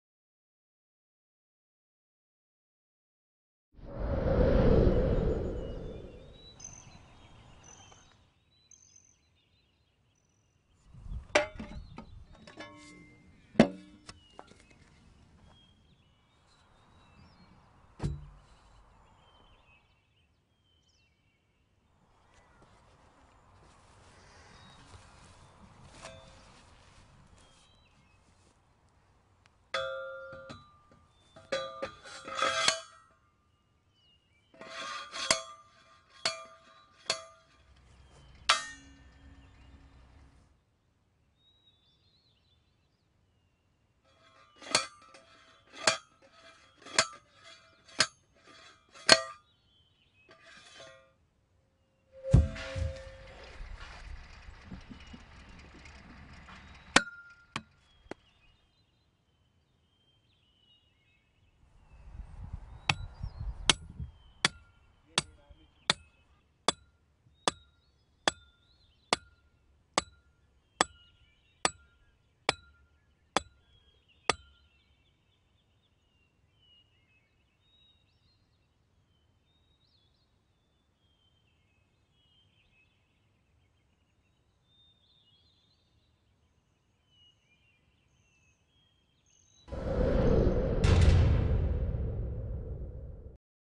How to - Waratah Ezypipe strainer post
A round steel strainer post alternative to traditional timber, pipe and concrete strainer posts. Quick to install, consistent quality & permanent strainer post solution. Three sizes: 2.15m, 2.50m and 3.25m lengths.